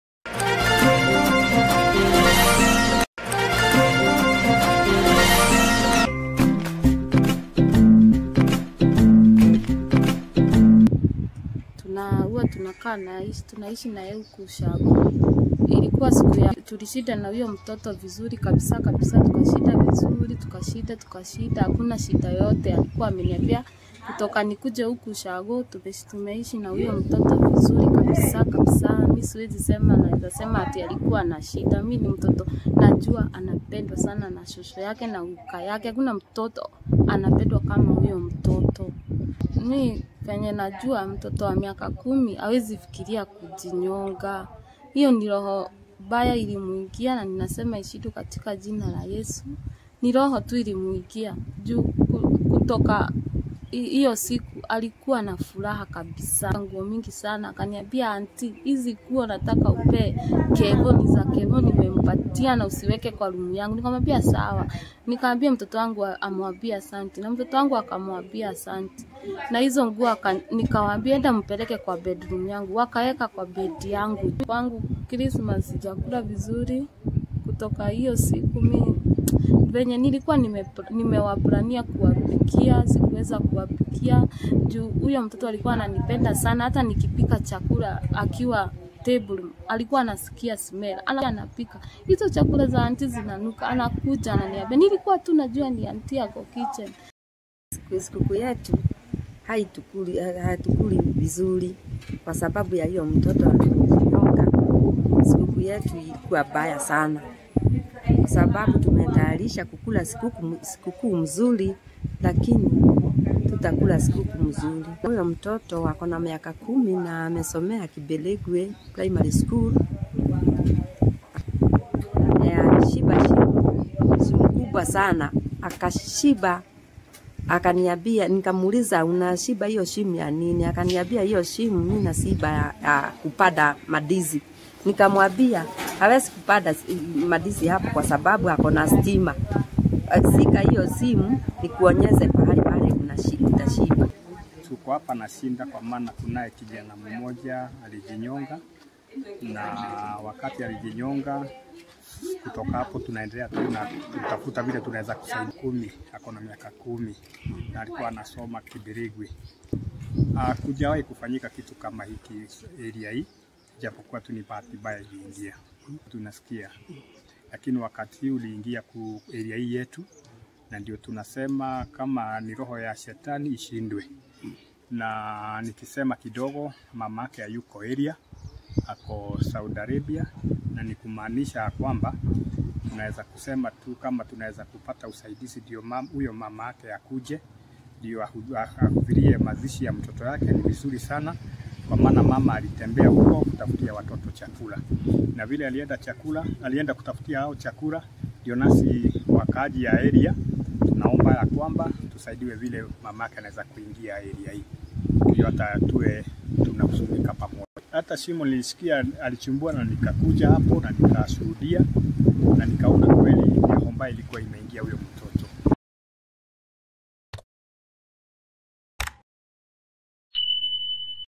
A FAMILY ENDS THE YEAR WITH CHILD COMMITTING SUICIDE
Shock and grief has engulfed Kibingoti village in Ndia, Kirinyaga County after a grade four pupil at Kibirigwi primary school died by suicide under unclear circumstance.

grandmother Rose Nyambura Ritho the 10-year-old boy took his life by hanging himself inside their house a few days after preparing his own grave.

 Nyambura said the boy had prepared his own grave and when asked said it was for planting bananas

Village elder John Mwangi said this is the first incident in Kibingoti village and the surrounding areas.